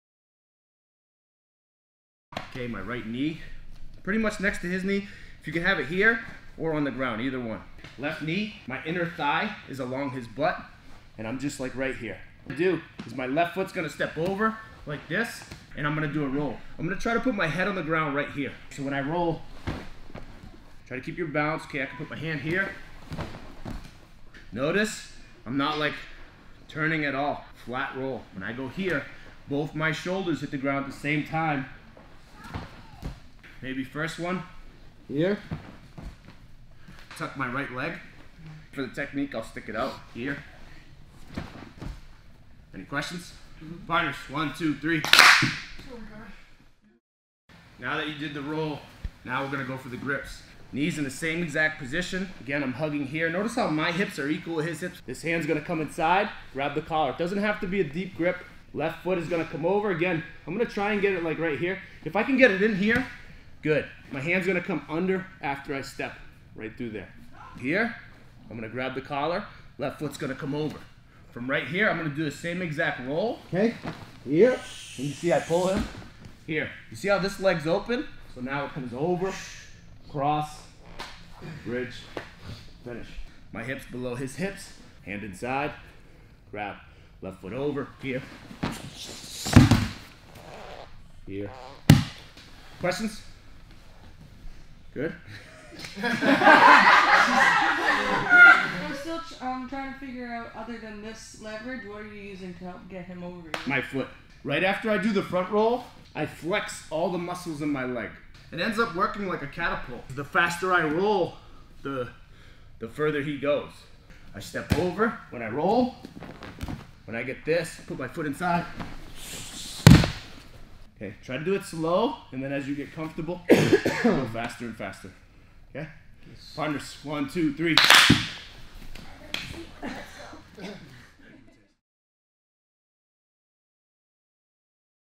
Rolling Bow and Arrow - Turtle Position
An excellent option for attacking an opponent in turtle position. This technique can also be executed on an opponent that is kneeling or reaching for your lead leg. More information can found in the blog post here:

One Love,

Professor Joseph

Academy Address:
The Paradigm Academy
109 Industrial Ave E
Unit 3